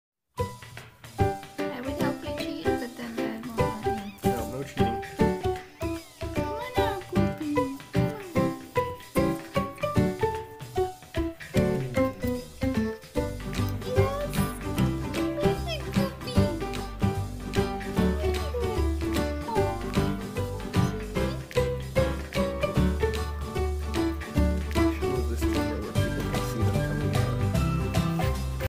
Exiting the Box, Part 3
Coopi leaves the box to visit DJ.